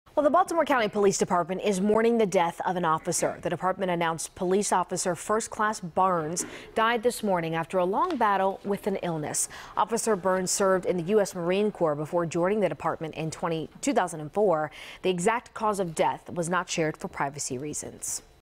Baltimore County Police Officer Byrnes Dies After Battle With Illness
A Baltimore County police officer died earlier this week after battling an unspecified illness, officials said Friday.